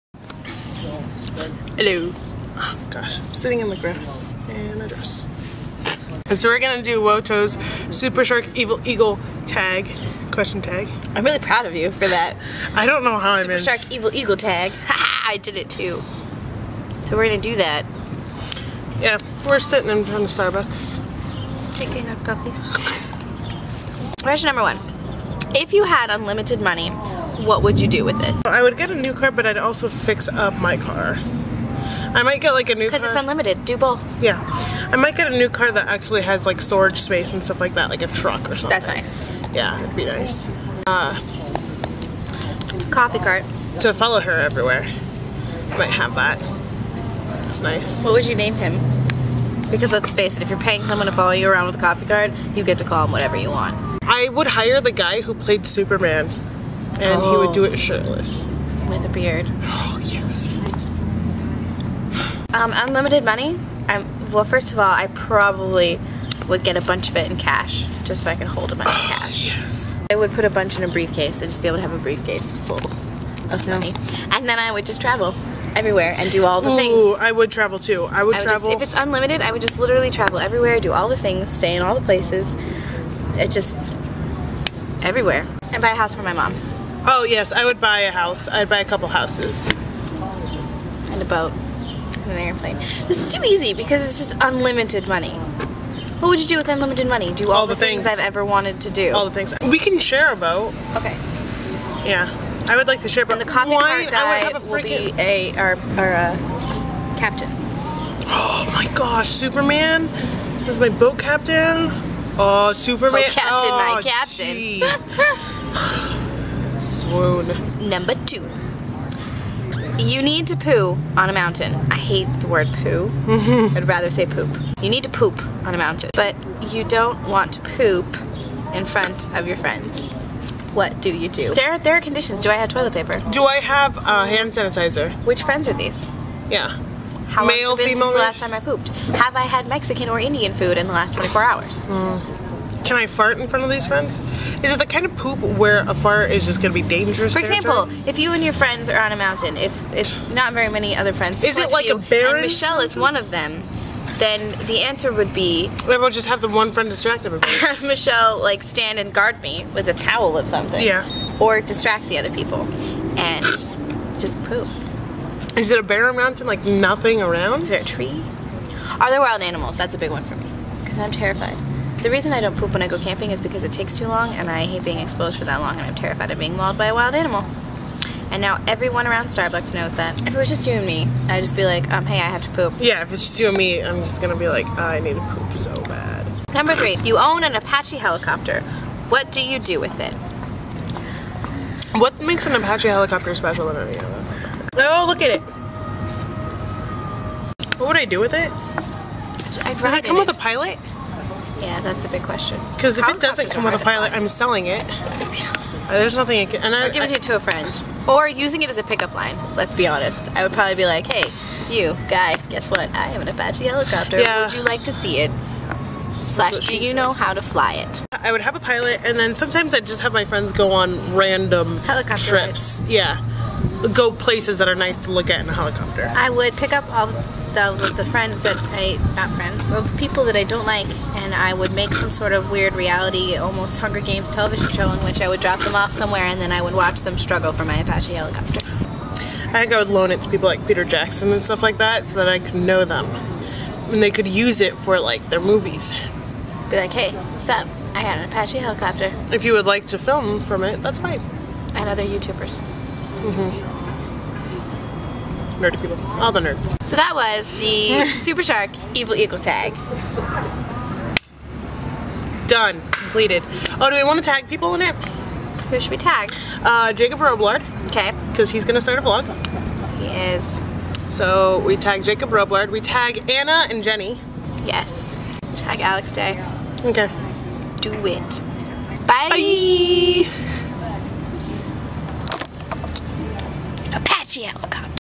WOTO Super Shark Evil Eagle Tag
This is Kelly and I's response to WOTO's Super Shark Evil Eagle Tag video.